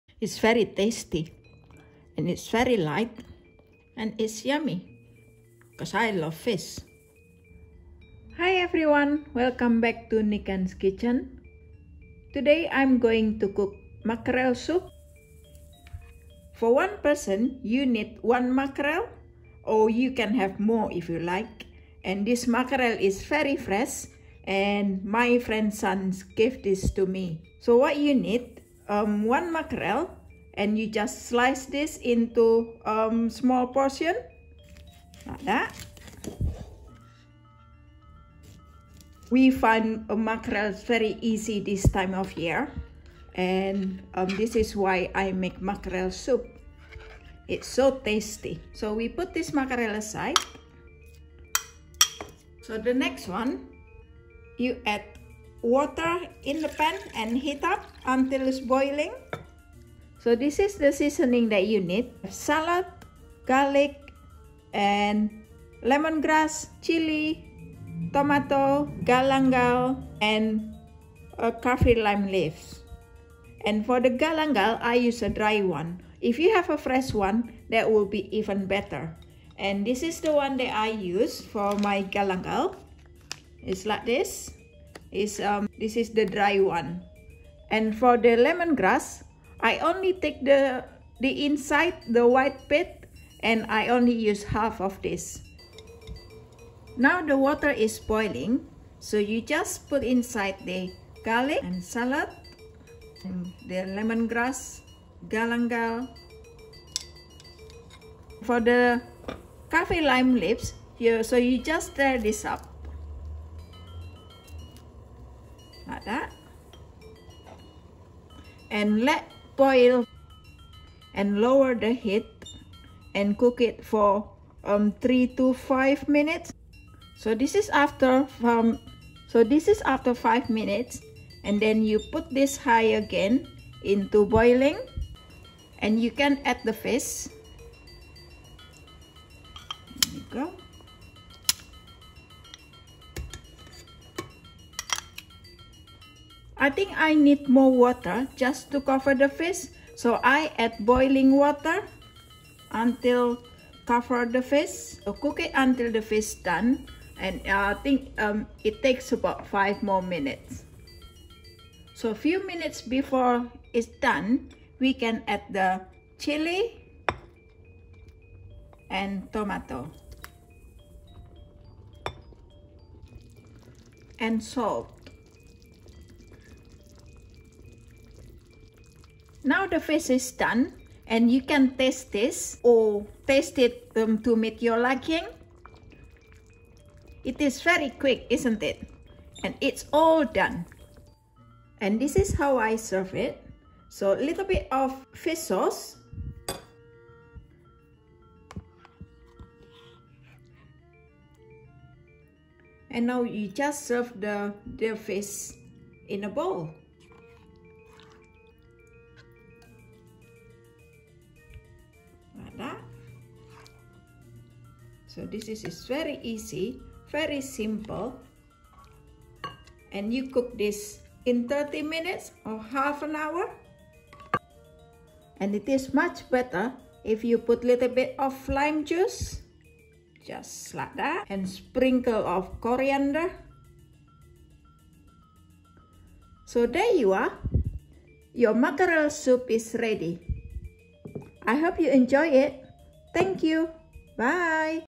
Mackerel Soup Recipe, Resep Sup Ikan, Fresh, Easy & Tasty!
This time of year here in UK, mackerels are easy to find. It's one of many recipes, mackerel soup, very easy & quick and perfect for lunch!

Ingredients :
1 - 2 Mackerels / Makarel, Tongkol, Patin, dll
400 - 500 ml Water / Air
1 Shallot, Sliced / B. Merah Iris
1 Garlic, Sliced / B. Putih Iris
1 Chilli, Sliced / Cabe Sesuai Selera
1 Tomato, Seeded, Sliced / Tomat Tanpa Biji, Iris
1 Slice Dried / Fresh Galangal / Langkuas
2 Kaffir Leaves / Daun Jeruk
Salt to Taste / Garam Sesuai Selera

To Serve :
Fish Sauce / Kecap Ikan/Asin
Slice Lime / Jeruk Nipis
Chopped Coriander / Daun Ketumbar Kalau Ada